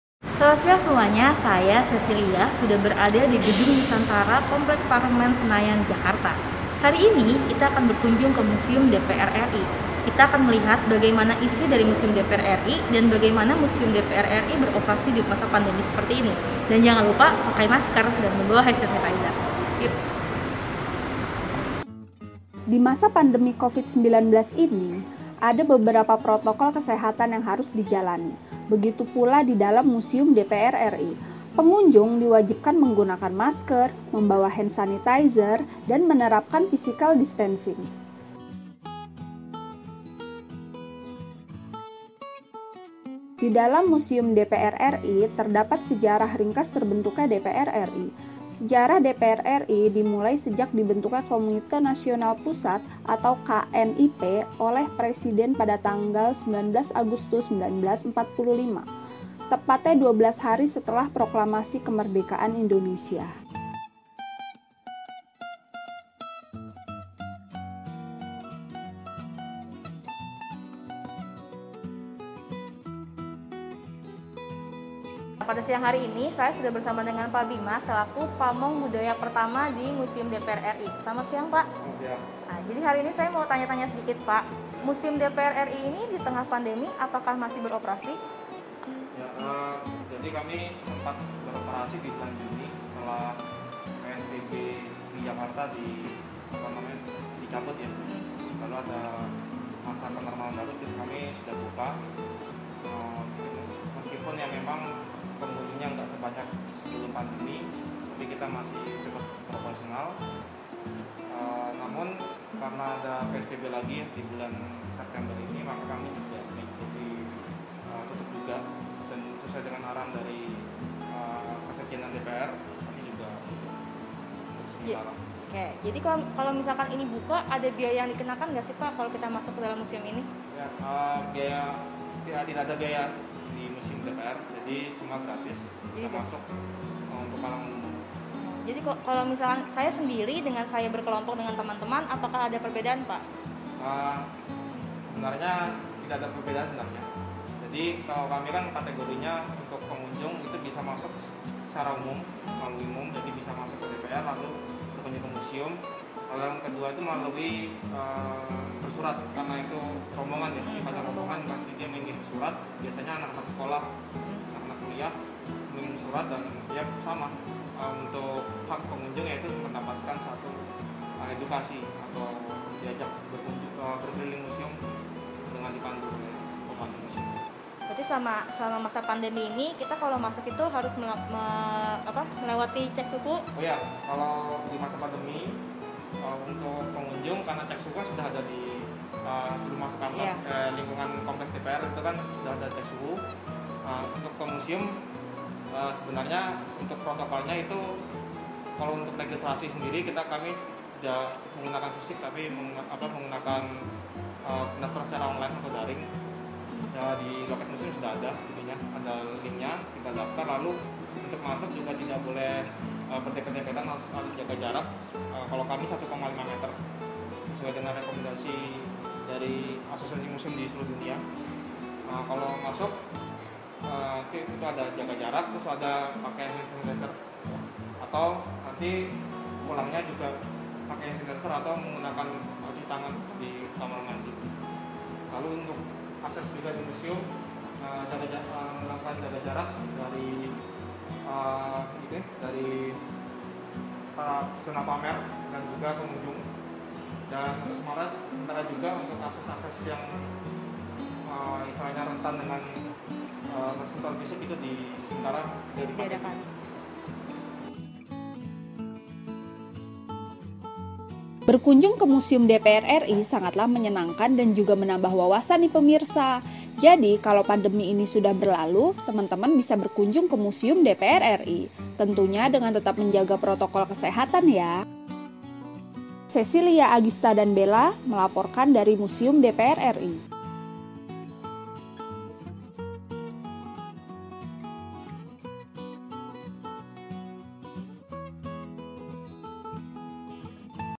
TUGAS AKHIR MAGANG TVR PARLEMEN : MUSEUM DPR-RI SELAMA PANDEMI
Reporter, penulis naskah : Cecilia Elizabet
Kameramen : Agista Puspa
Editor, VO : Bella Karina